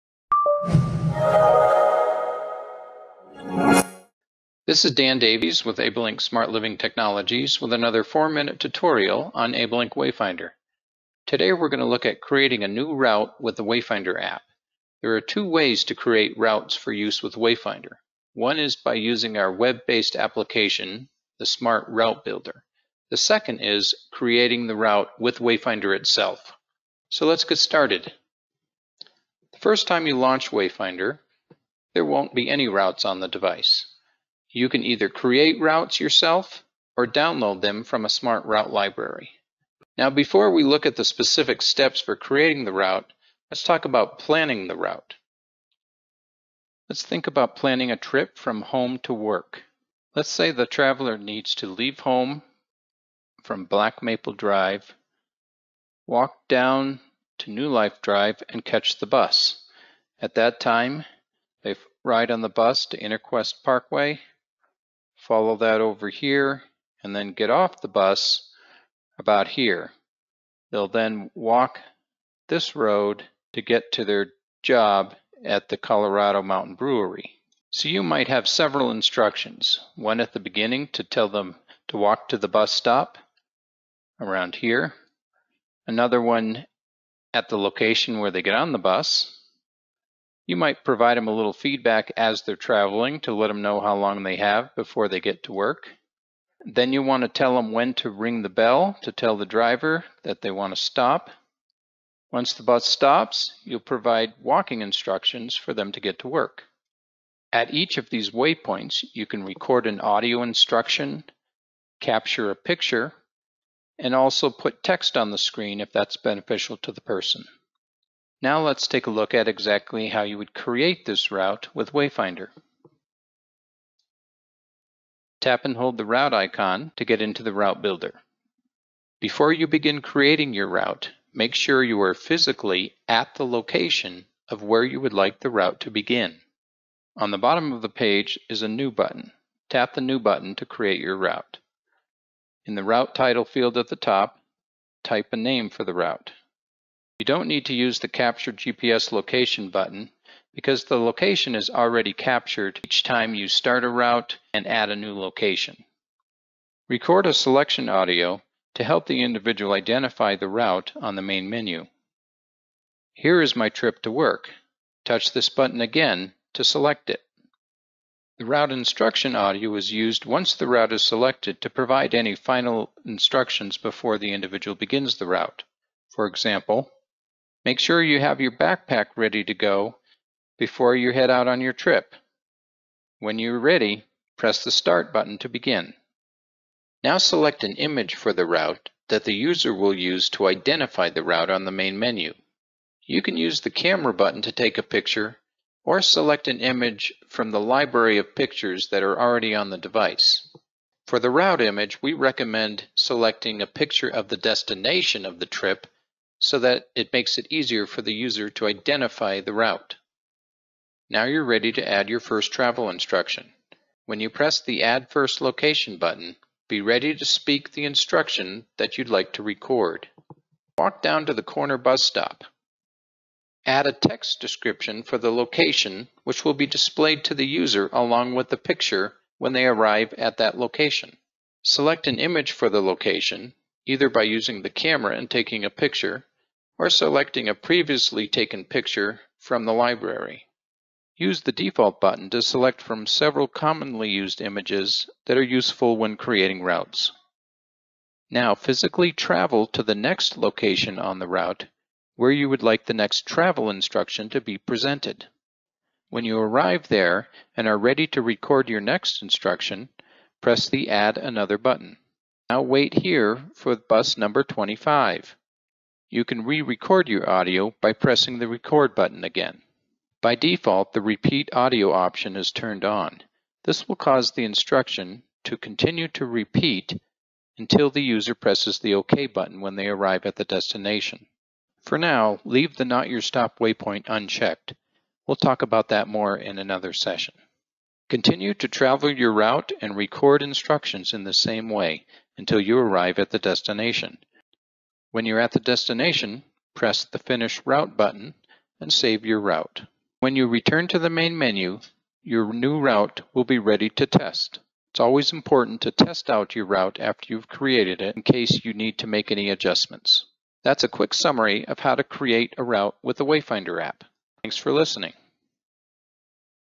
AbleLink 4-Minute Tutorial #1 - WayFinder: Creating a Route with the WayFinder App
This tutorial shows how to create a SMART travel route with the WayFinder App.